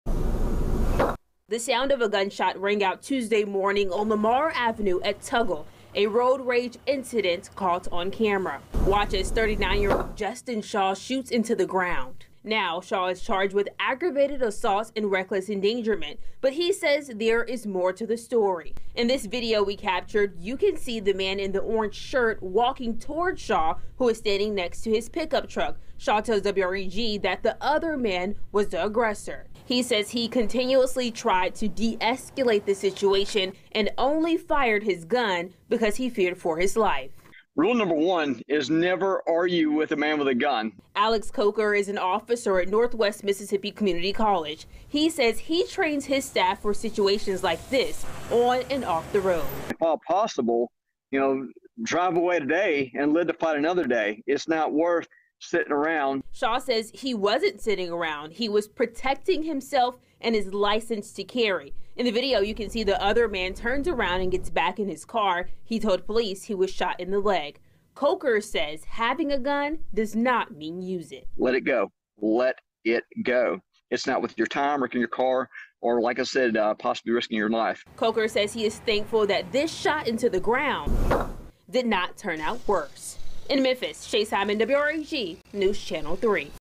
Road rage shooter says he feared for his life
Earlier this week, WREG told you about a road rage shooting on a Memphis street that ended with a man in jail. Now, that man says although he was the one arrested, there is more to the story.